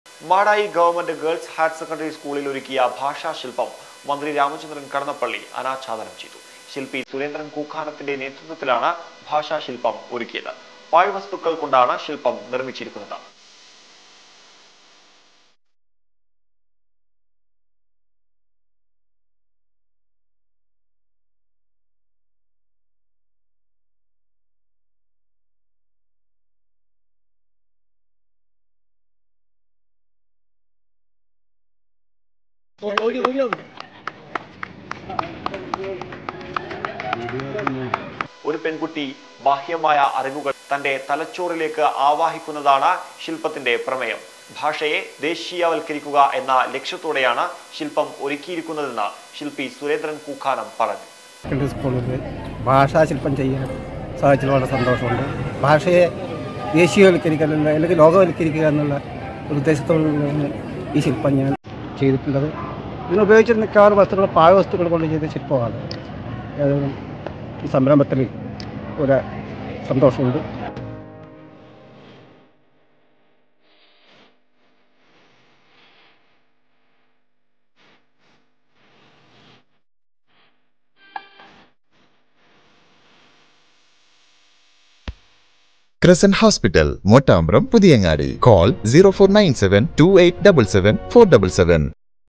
ഭാഷാ ശില്പം മന്ത്രി രാമചന്ദ്രൻ കടന്നപ്പള്ളി അനാച്ഛാദനം ചെയ്തു...
മാടായി ഗവൺമെന്റ് ഗേൾസ് ഹയർ സെക്കൻഡറി സ്കൂളിൽ ഒരുക്കിയ ഭാഷാ ശില്പം മന്ത്രി രാമചന്ദ്രൻ കടന്നപ്പള്ളി അനാച്ഛാദനം ചെയ്തു...